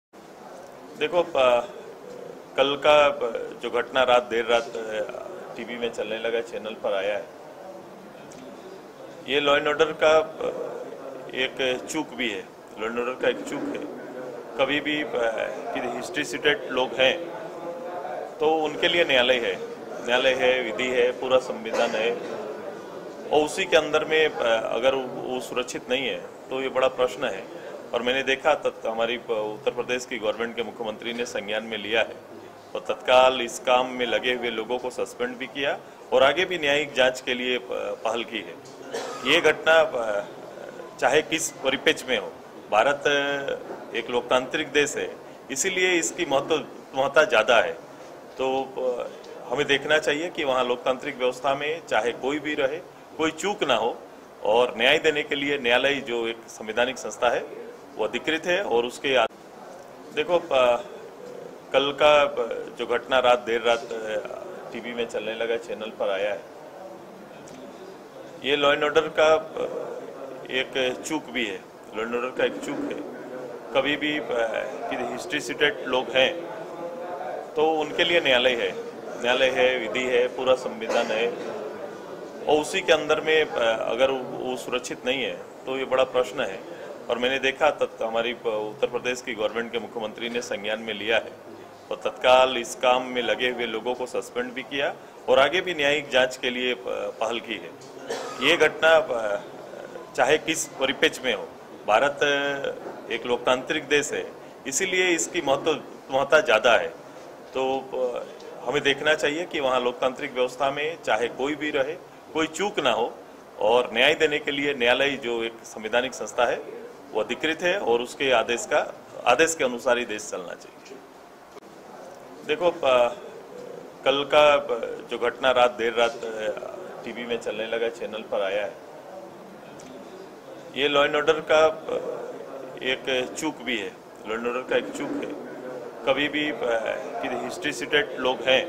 UP में कानून व्यवस्था की स्थिति, गैंगस्टर अतीक की पुलिस कस्टडी में हत्या पर क्या बोले सुदेश महतो
धन्यवाद।